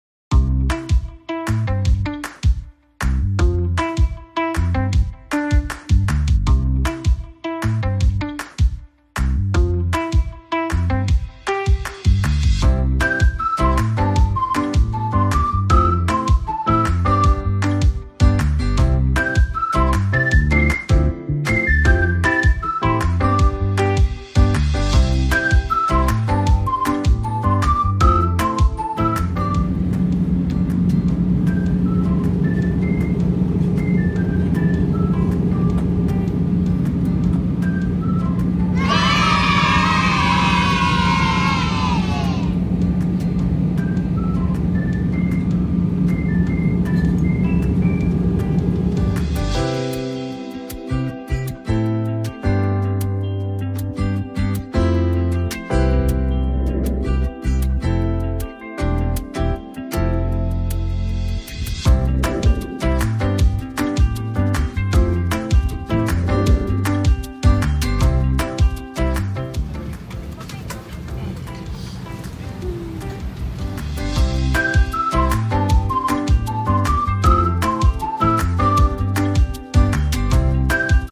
MUNTIK NA MAIWAN NG AIRASIA SI RAYLEIGH GIN!!!
Rayleigh Gin muntik maiwan ng flight!!